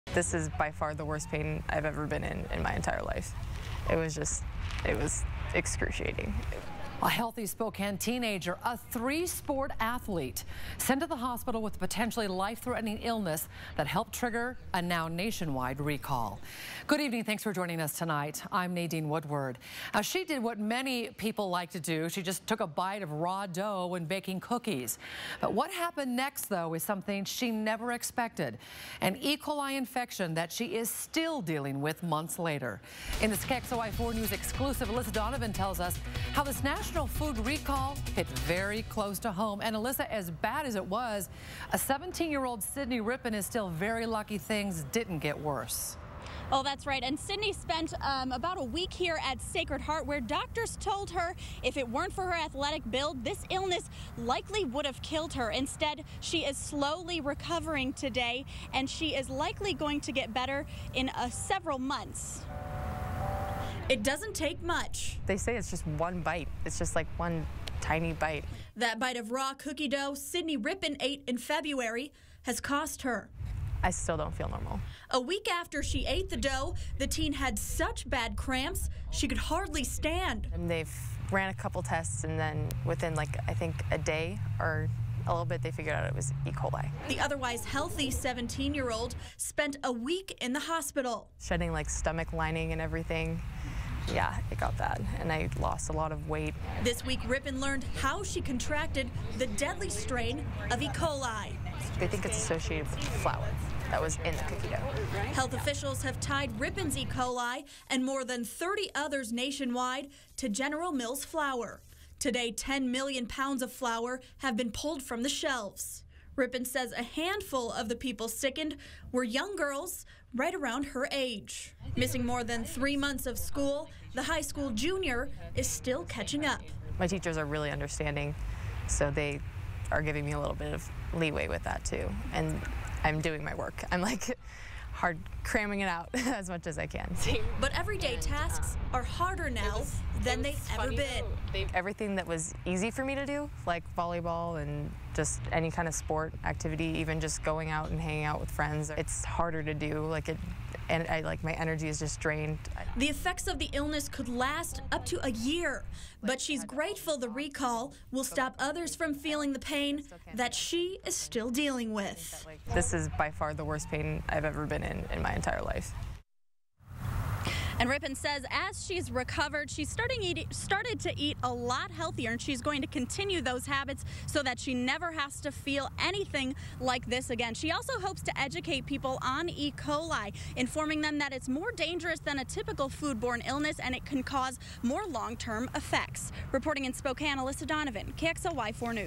One bite of cookie dough left Spokane teen fighting for life
Sydney Rypien was a healthy Spokane teenager and a three-sport athlete.  Then she took a bite of raw cookie dough and ended up in the hospital  soon afterward fighting for her life. KXLY4's Alyssa Donovan reports.